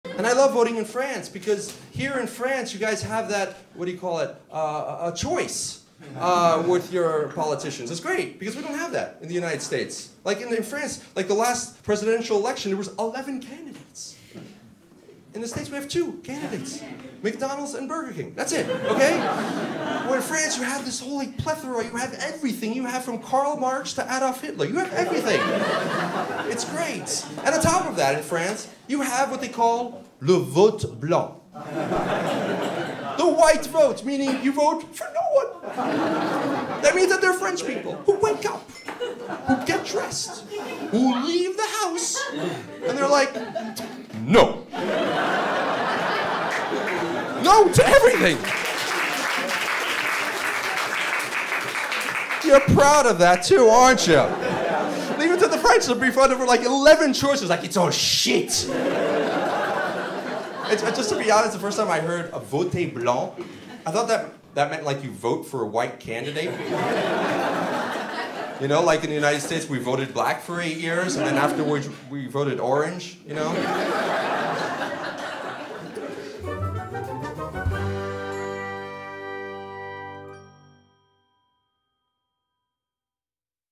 Voting in France
What's nice about voting in France, as opposed to the United States, is that you you have a choice!
Filmed at the Complexe Du Rire in Lyon.
English and French subtitles available.